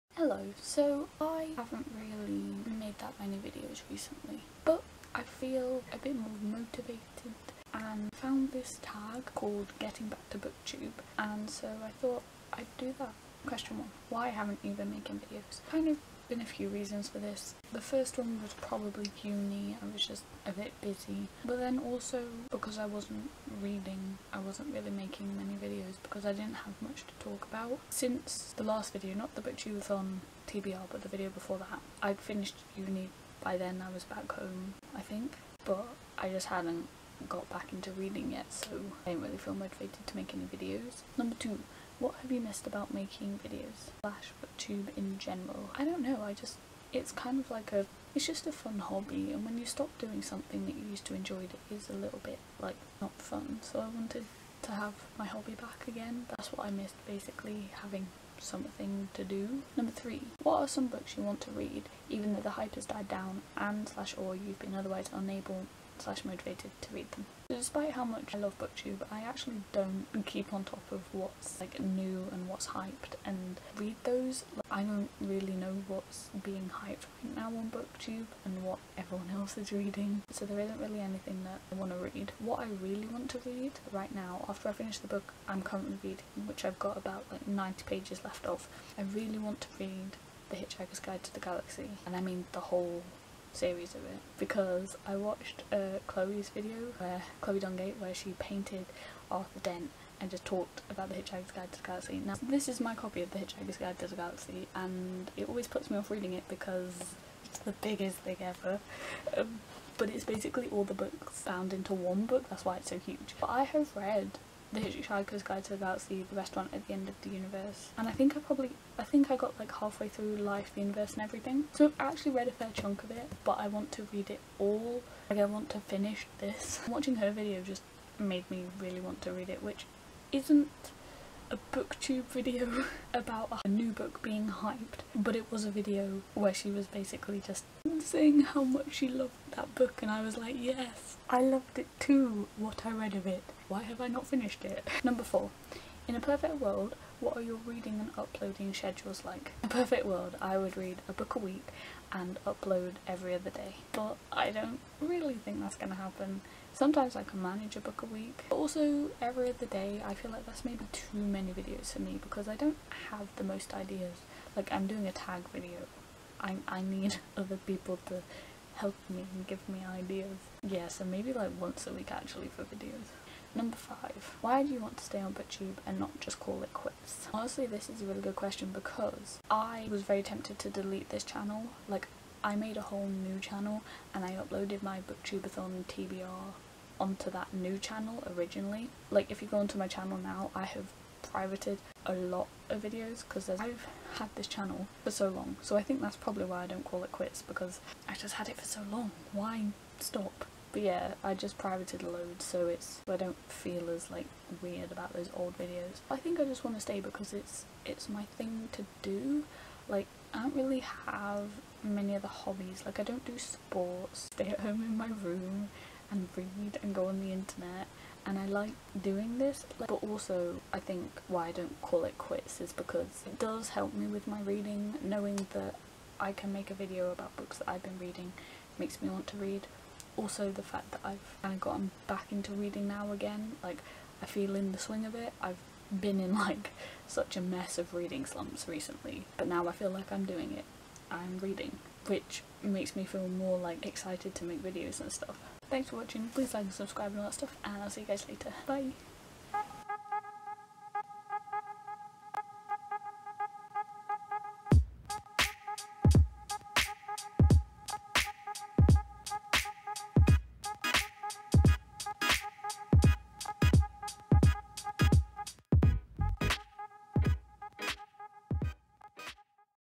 Getting Back to Booktube
Hello. So I haven't been the most active on here recently, but I want to get back to booktube and I found a tag with questions that let me explain why I'm sticking to making videos.
The Questions:

1. Why haven't you been making videos?

2. What have you missed about making videos/booktube in general?

3. What are some books you want to read, even though the hype has died down and/or you've been otherwise unable/unmotivated to read them?

4. In a perfect world, what are your reading and uploading schedules like?

5. Why do you want to stay on booktube and not just call it quits?